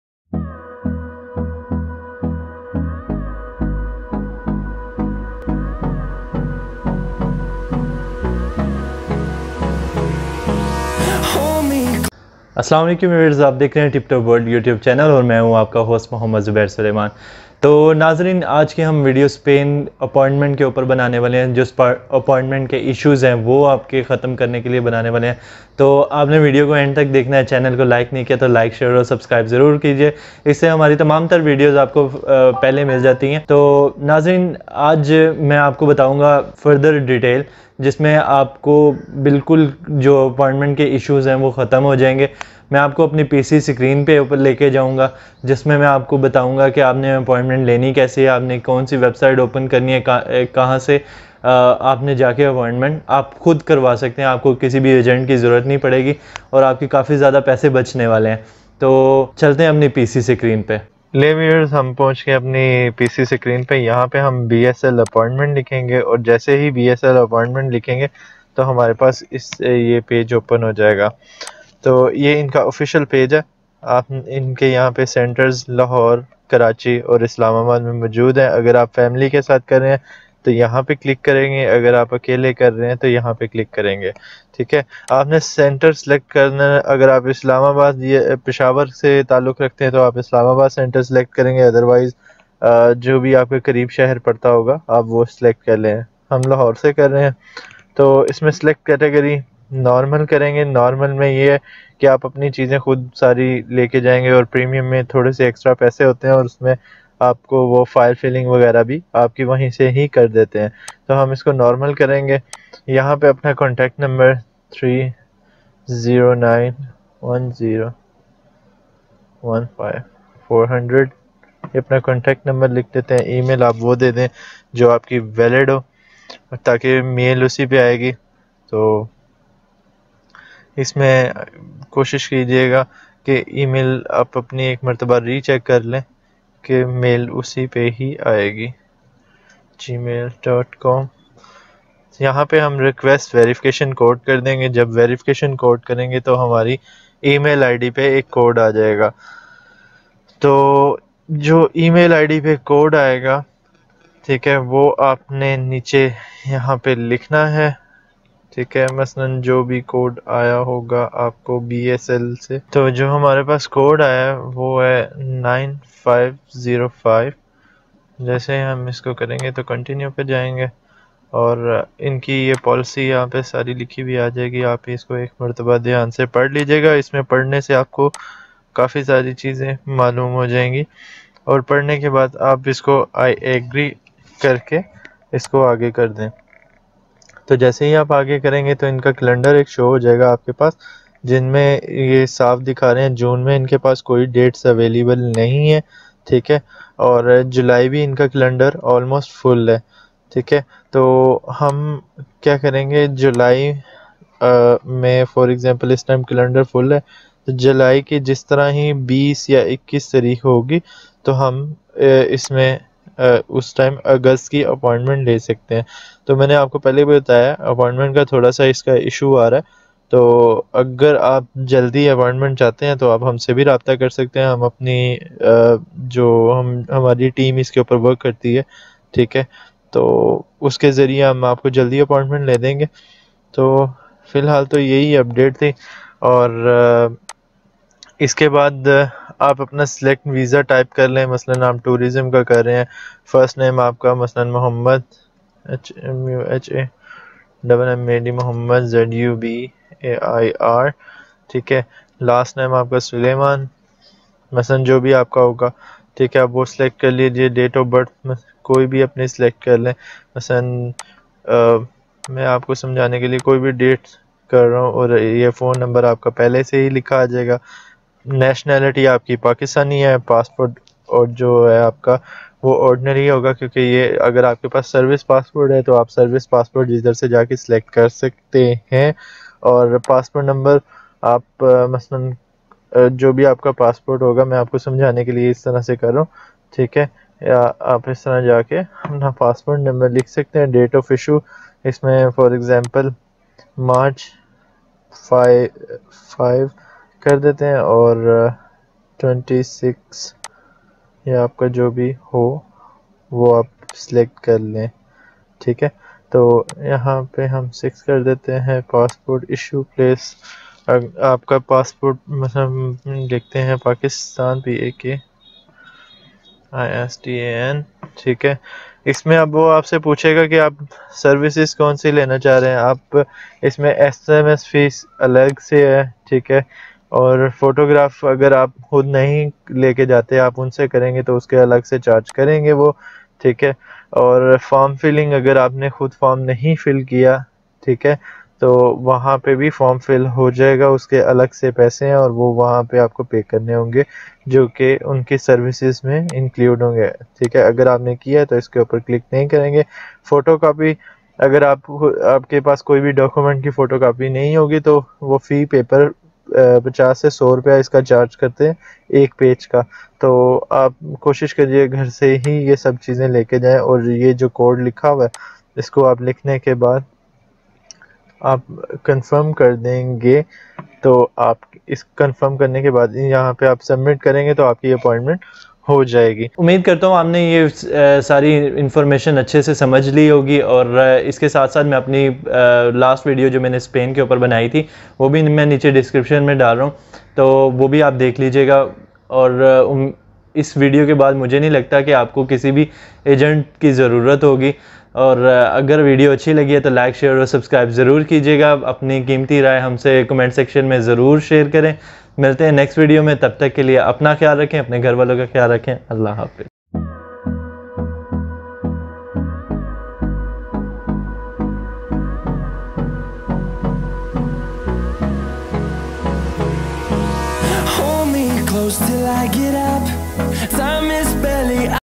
How to Get Spain Visa II Easy Way to get Appointment II Full Detail
Viewers In this video I will tell you and guide you about very Good News about Spain Visa. As Spain government opens visit Visa for everyone from 14-June-2021 and in  this Video I will tell you how to get appointment without any help of Agents .

Stay with Us.. For Personal Assistance please contact on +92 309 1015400

Tiptop786_world